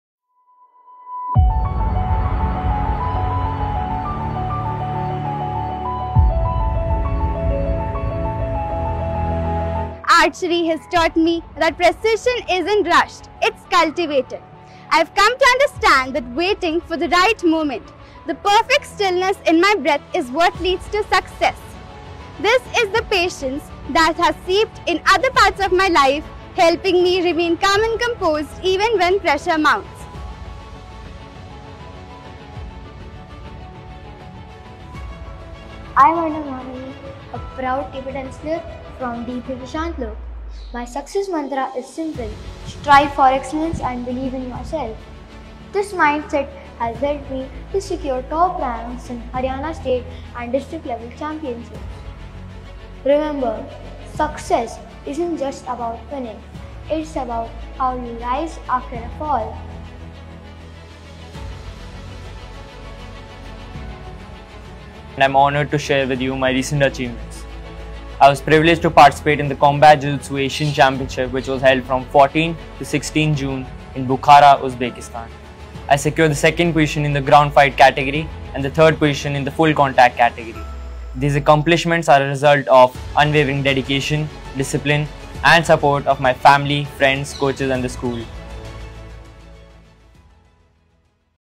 Delhi Public School, Sushant Lok is proud on achievements in sports.
Celebrating success and perseverance! 🎉

Delhi Public School, Sushant Lok is proud on achievements in sports. Our students continue to shine bright, showcasing their skills, determination, and passion in every field they pursue. 🌟

Each milestone brings us closer to our shared vision of holistic development and lifelong learning.

Kudos to our champions for making us proud once again!